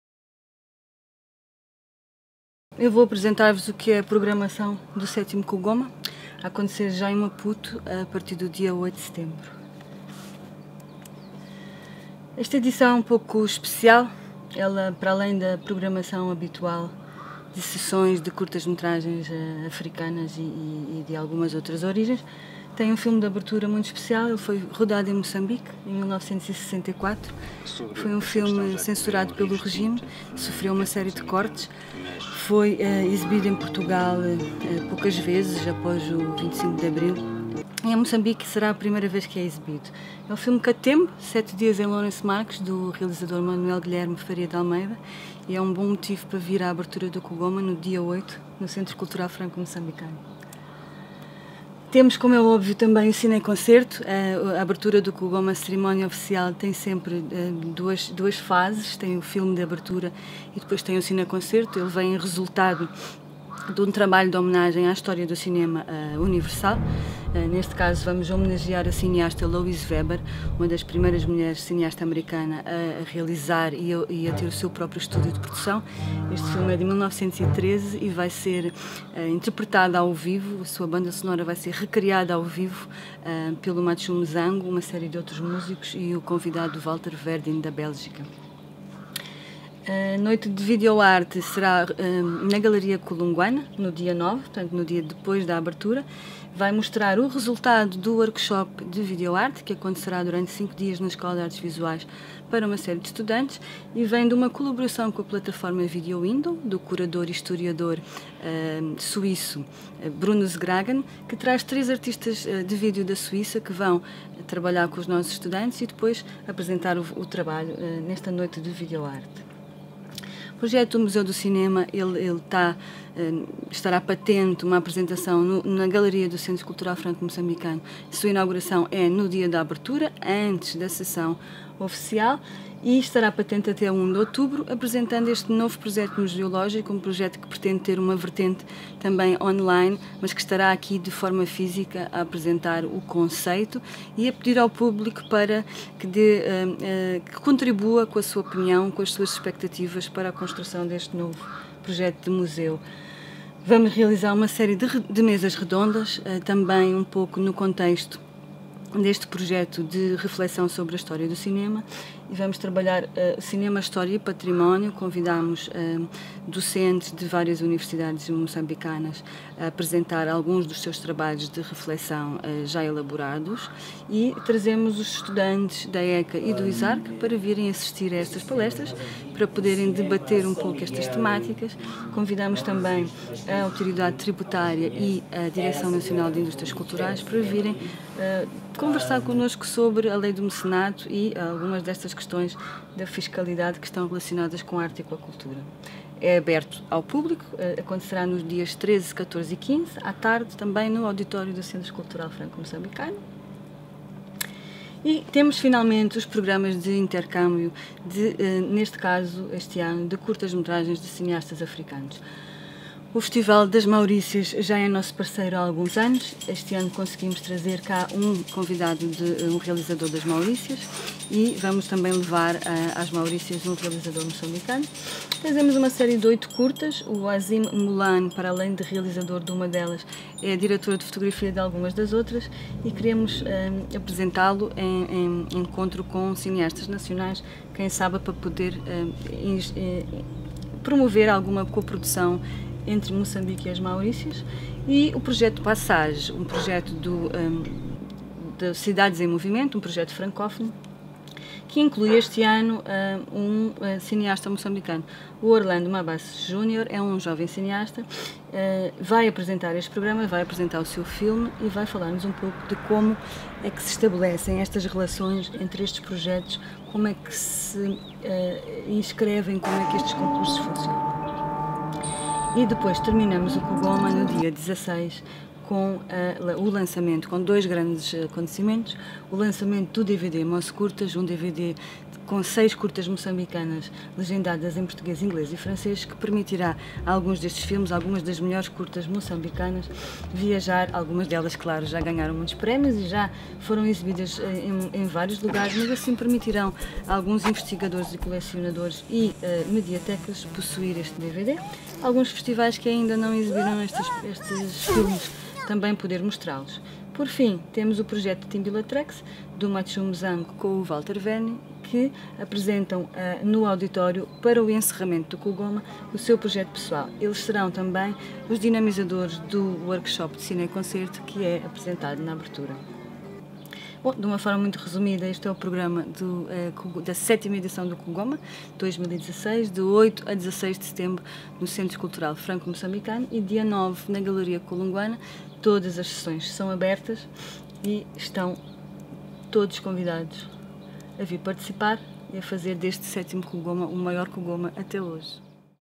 Programa KUGOMA 2016
A produtora e Coordenadora do KUGOMA, Diana Manhiça, fala-nos da programação da 7.ª edição do evento.
KUGOMA's producer and coordinator, Diana Manhiça, tells us about the event's 7th edition program.